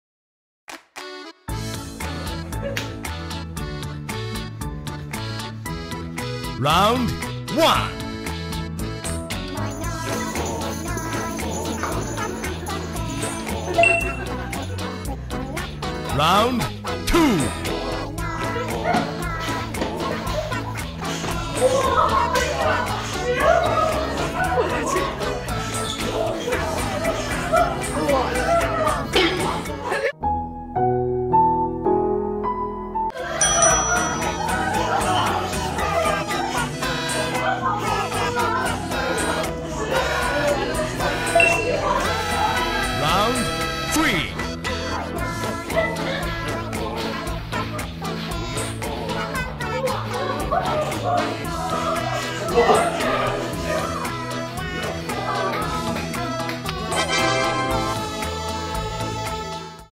Blowing Paper Towel——Keep the Tissue in the Air
Let's compare vital capacity together.